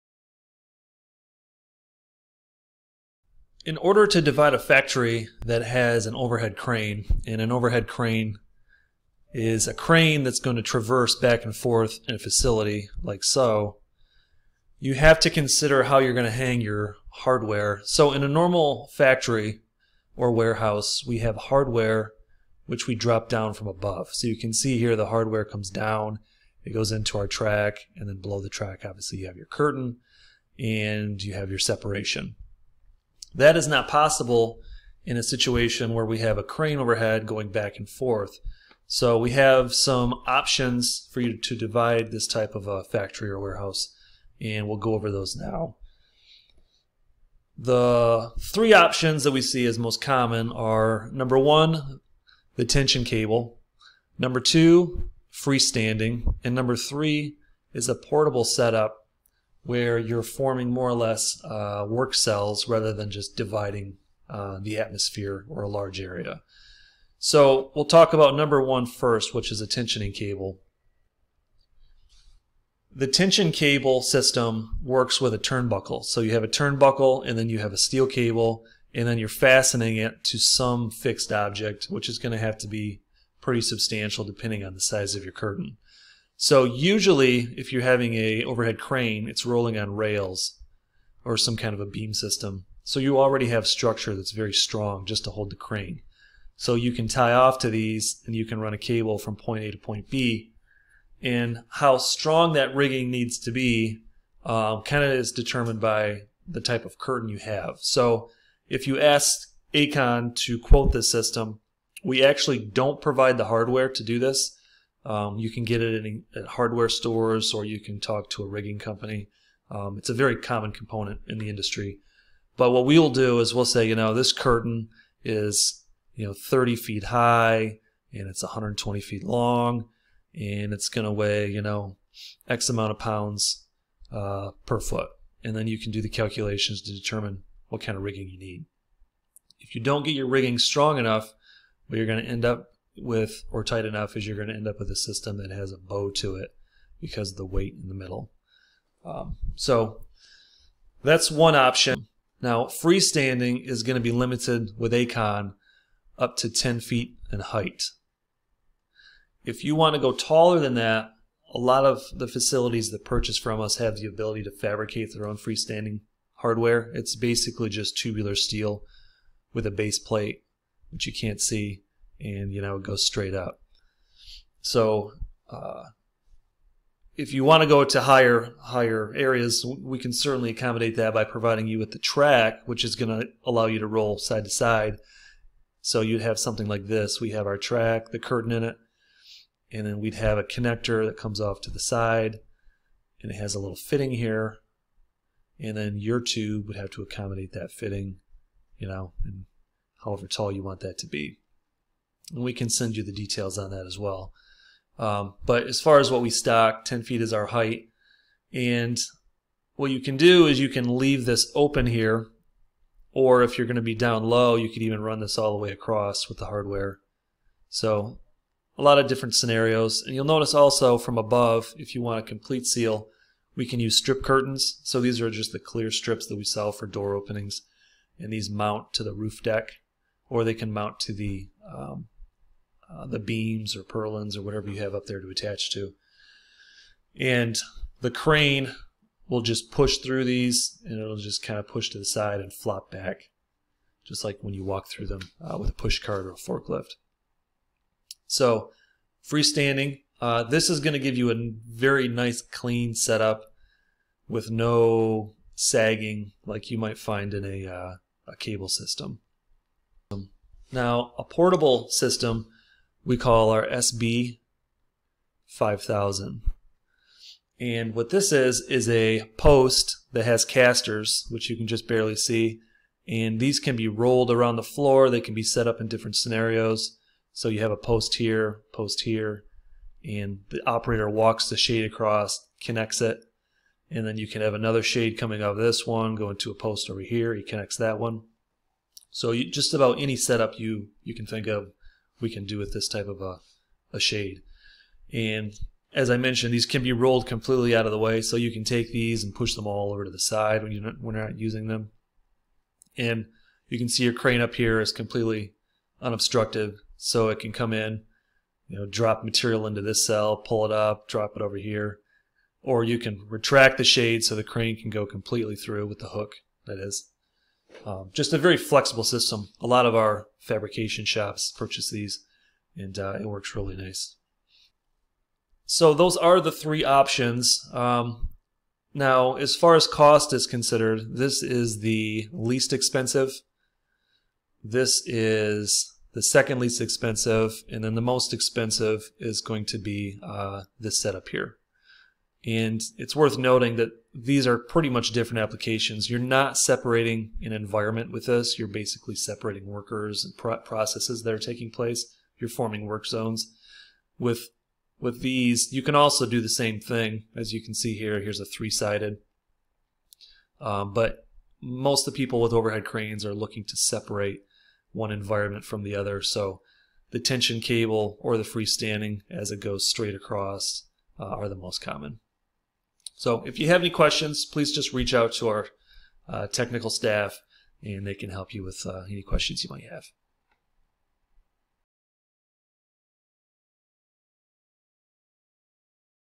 Dividing a Factory With an Overhead Crane
How to divide your factory when you cant support a curtain wall system from above. This video shows how our warehouse partitions can be used with a cable system from one beam to another.